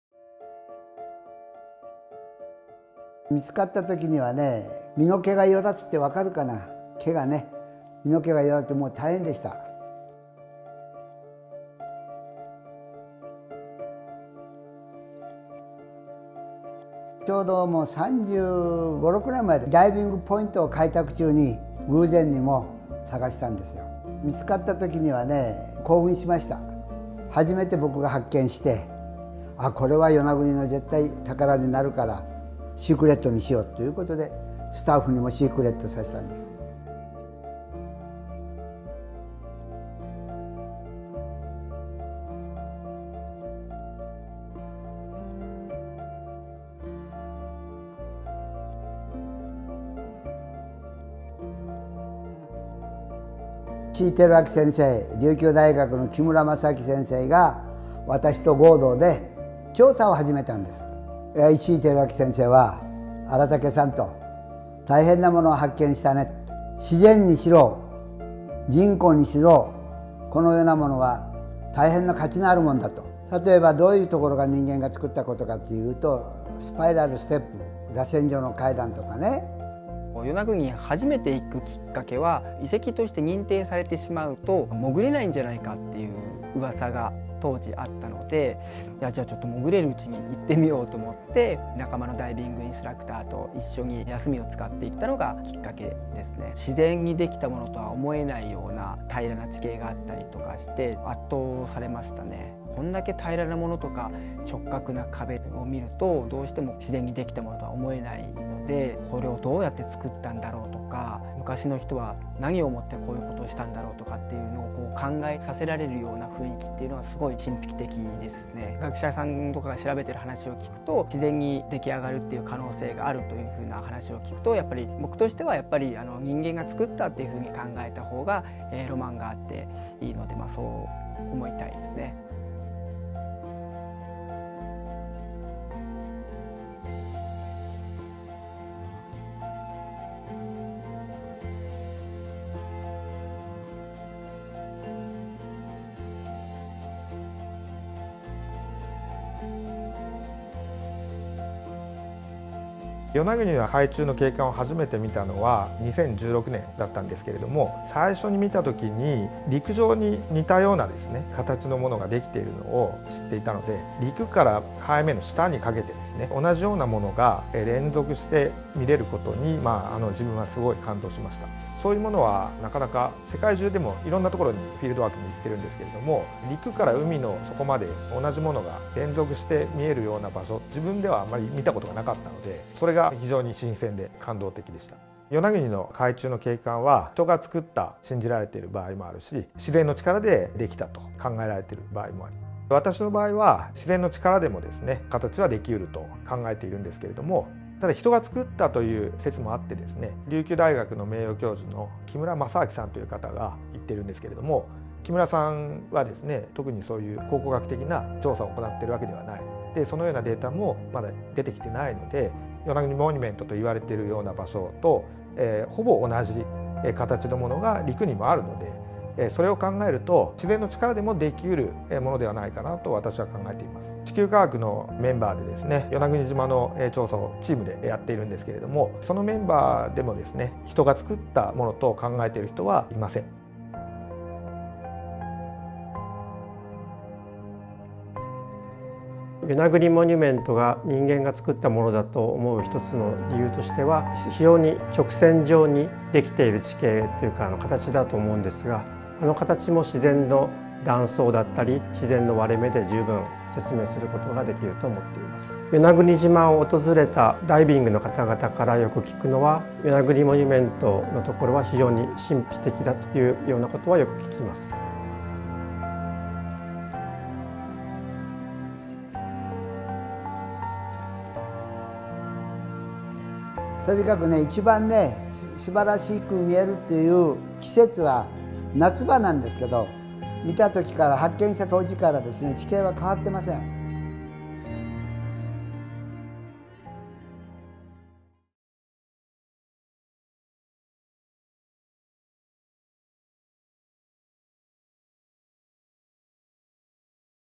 The truth behind Japan's mysterious 'Atlantis' – BBC REEL
Beneath the coastal waters of the Japanese island of Yonaguni lies a series of mysterious rock formations. Since its discovery nearly 35 years ago, the strangely symmetrical shapes and structures of the 'Yonaguni Monument' have led to fierce debate over its true origins.

Could the odd shapes of this structure really have been carved by a lost Japanese civilisation many thousands of years ago, or is it just a stunning example of natural geology deep beneath the sea?

Video by Naotomo Umewaka